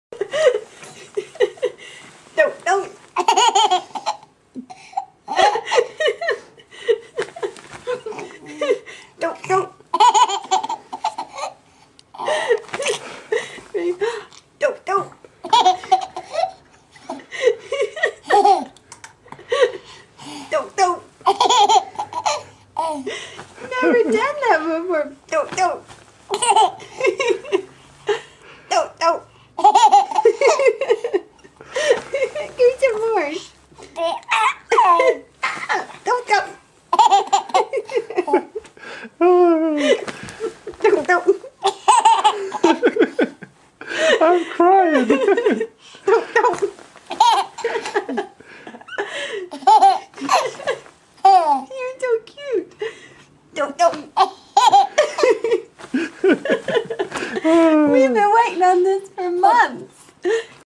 Mama's So Funny
He finally laughs out loud!!